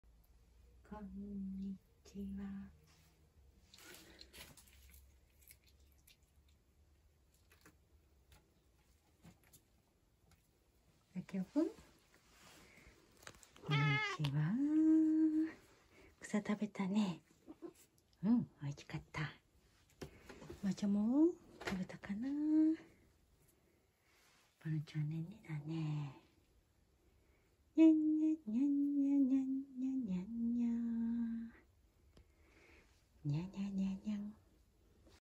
あいさつに来てくれるハチワレ猫ざきお😸くつろぐキジトラ猫マール🐈ねんねの黒猫バロン【猫動画cats】
猫草食べてくつろぐざきおくん、マールちゃん、バロンちゃん🐈🐈‍⬛